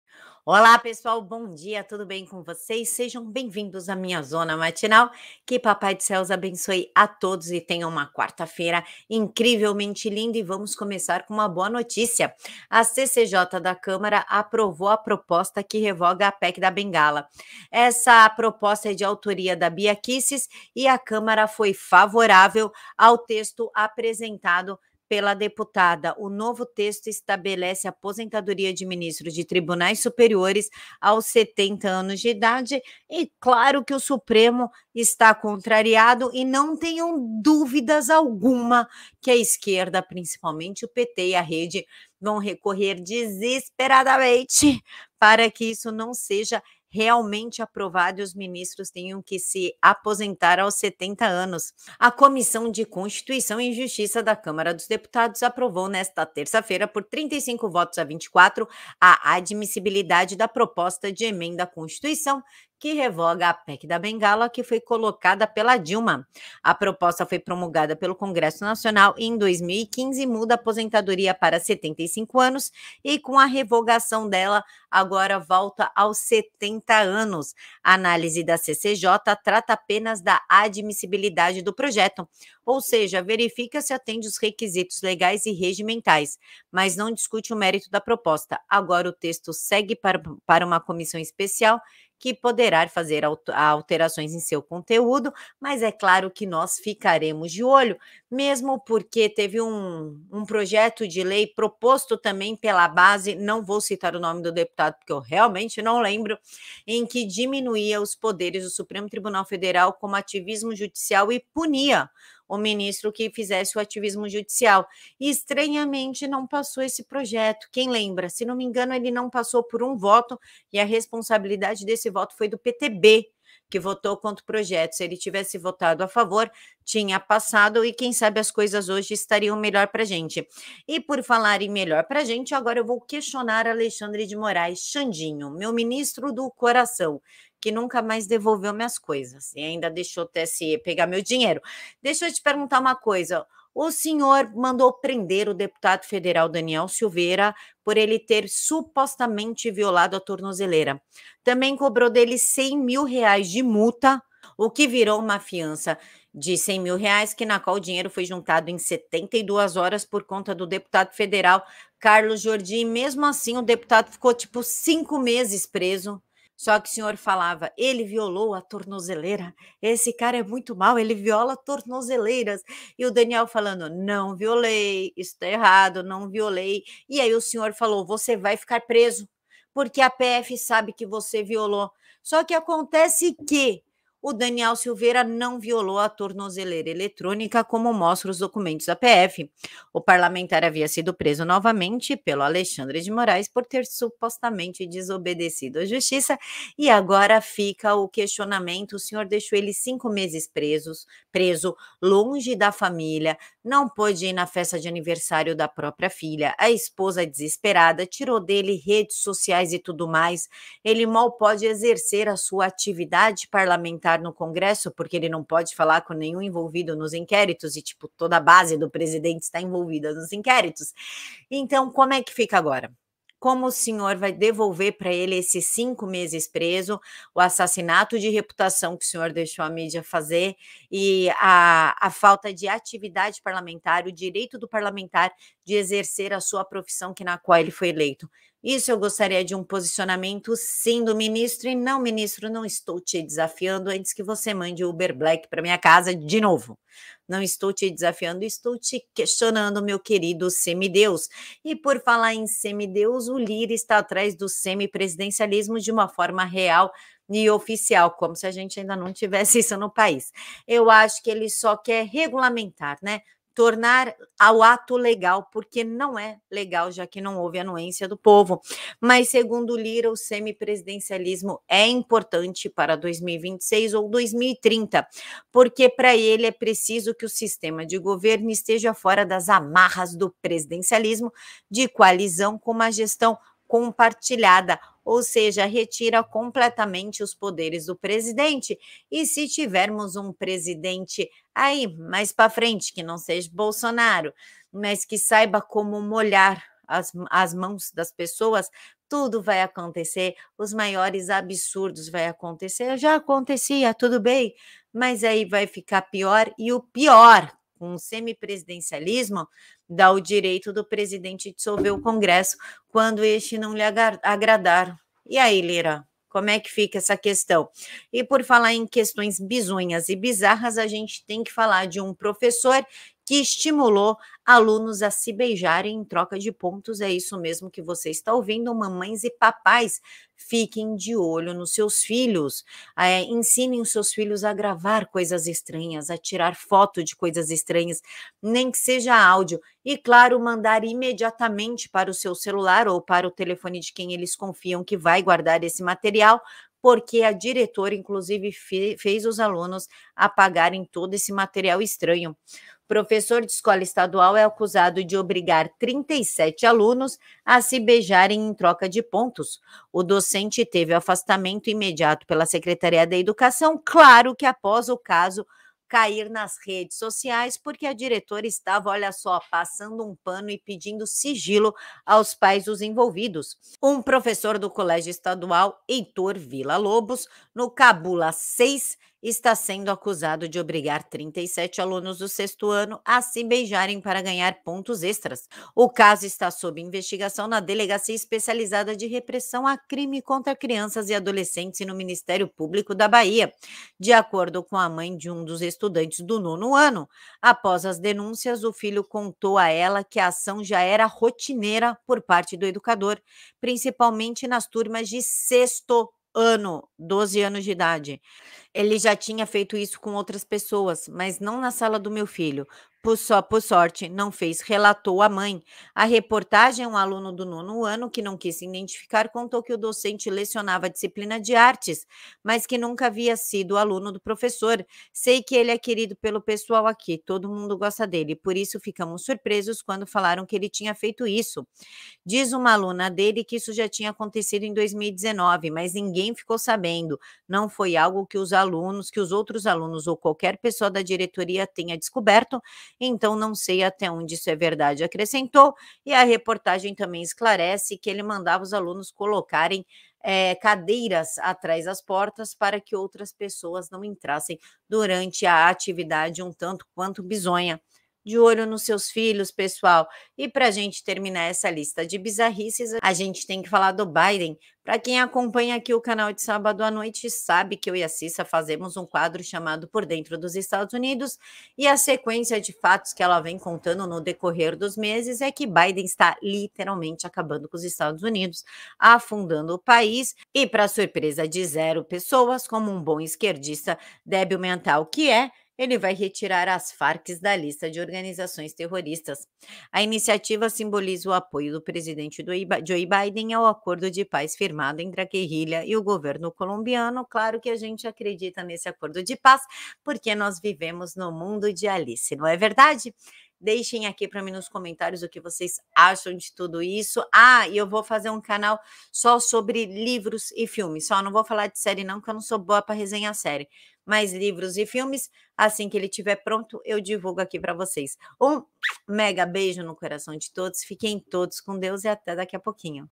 Daniel Silveira INOCENTE, Prof faz alunos se beijarem, Fim da PEC da Bengala, E+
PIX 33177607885
Correio da Bahia: Professor de escola estadual é acusado de obrigar 37 alunos a se beijarem em troca de pontos
Revista Oeste: Lira insiste em semipresidencialismo ‘para 2026 ou 2030’
Revista Oeste: Daniel Silveira não violou a tornozeleira eletrônica, mostra documento da PF
Revista Oeste: CCJ da Câmara aprova proposta que revoga PEC da Bengala